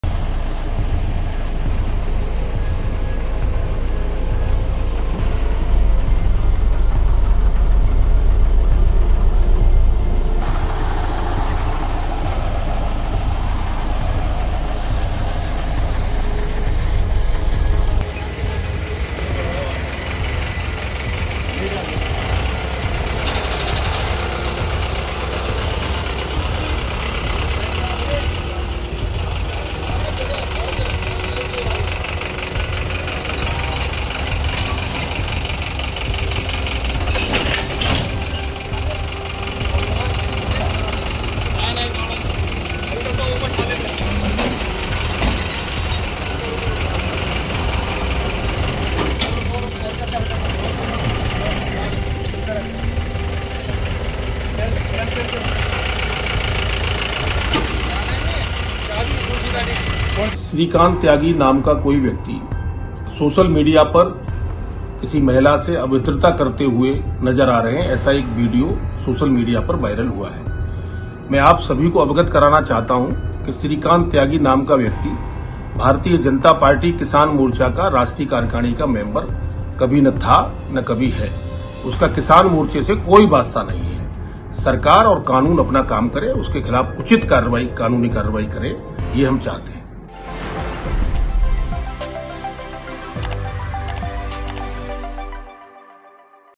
Noida: महिला को गाली देने वाले बीजेपी नेता की गाड़ियां सीज, घर पर छापा पड़ा तो हुआ फरार| NBT UP
नोएडा की सोसाइटी में अवैध कब्जे के नाम पर हुए विवाद में एक महिला के साथ अभद्रता करने वाले बीजेपी नेता पर सख्त ऐक्शन शुरू हो गया है। नोएडा के सेक्टर-93 स्थित बीजेपी नेता श्रीकांत त्यागी के घर पर पुलिस का छापा पड़ा है। बताया जा रहा है कि त्यागी घटना के बाद से ही फरार है और पुलिस टीमें उसकी तलाश कर रही हैं। श्रीकांत के तमाम लग्जरी वाहनों को पुलिस ने जब्त कर लिया है। इन वाहनों को क्रेन के जरिए पुलिस अब थाने लेकर पहुंची है। नोएडा में हुई इस कार्रवाई के बीच बीजेपी ने श्रीकांत से किसी भी तरह का संबंध होने से इनकार किया है। बीजेपी के किसान मोर्चा के राष्ट्रीय अध्यक्ष राजकुमार चाहर ने कहा है कि श्रीकांत त्यागी नाम का कोई भी शख्स कभी बीजेपी के किसान मोर्चा की राष्ट्रीय कार्यसमिति का हिस्सा नहीं था।